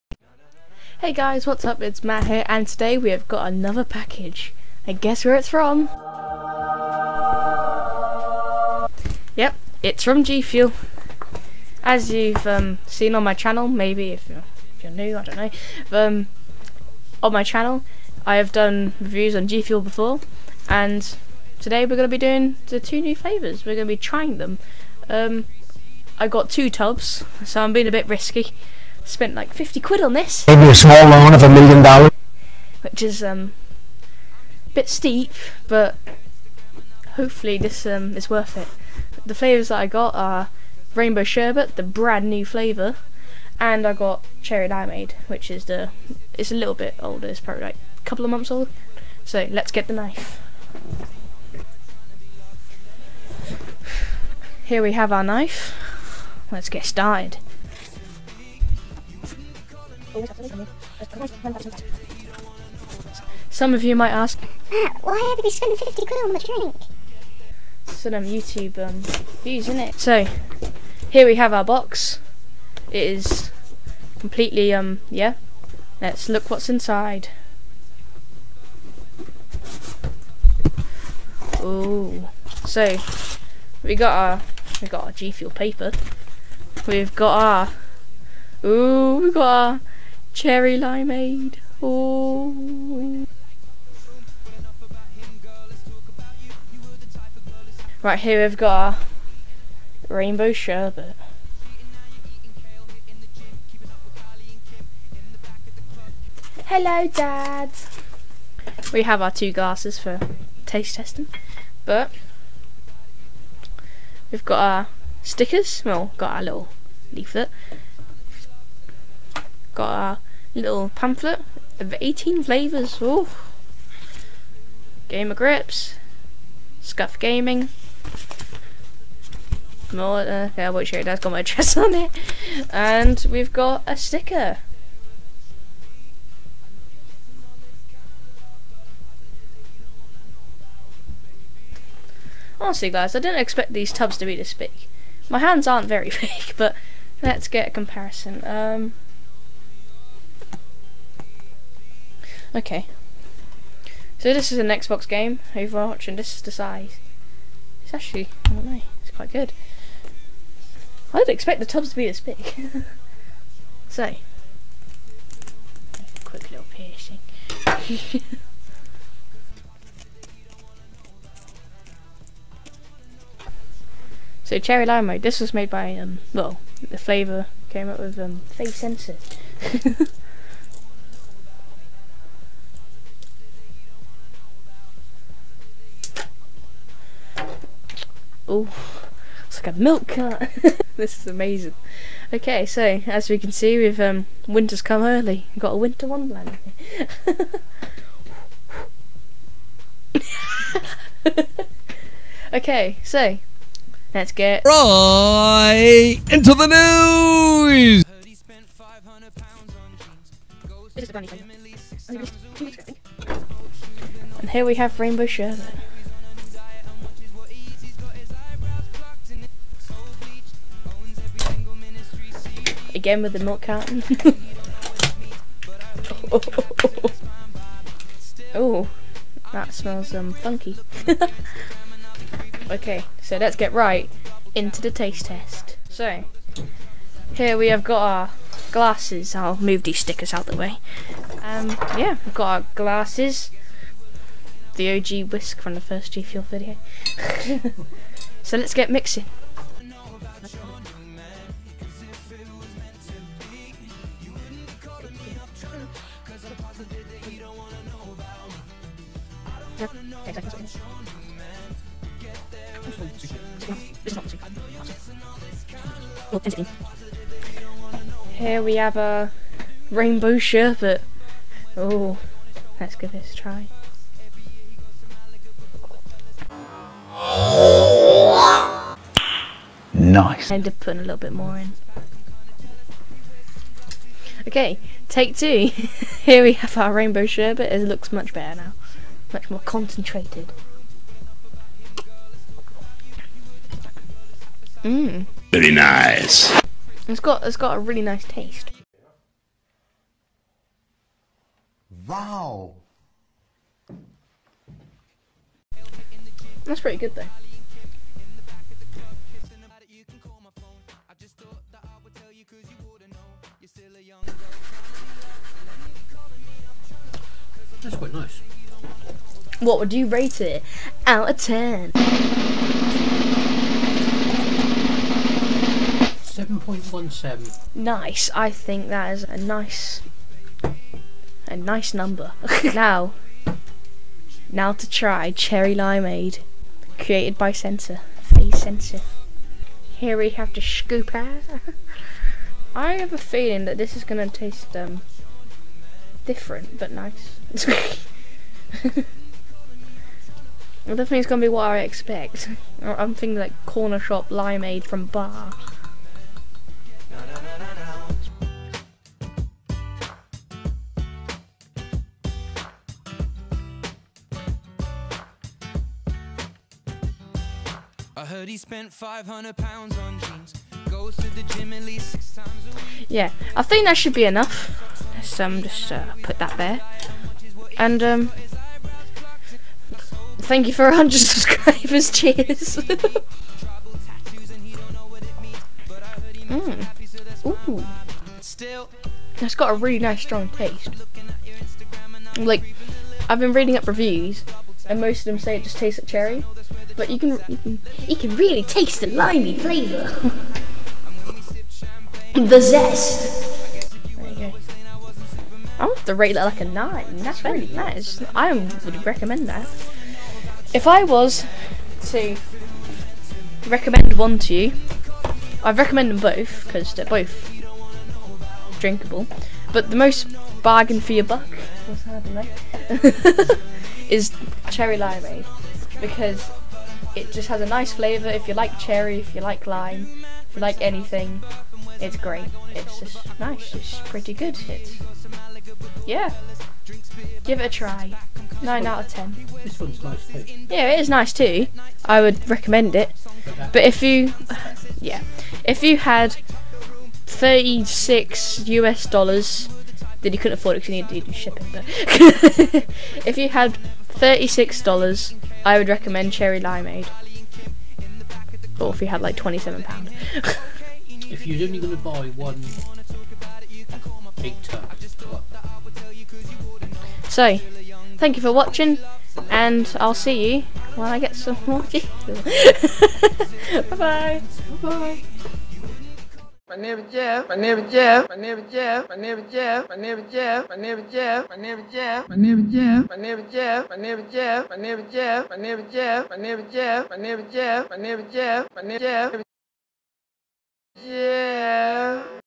TRYING RAINBOW SHERBET and CHERRY LIMEADE GFUEL (TASTE TEST!)
Today I try the TWO NEWEST GFUEL FLAVOURS, RAINBOW SHERBET and CHERRY LIMEADE!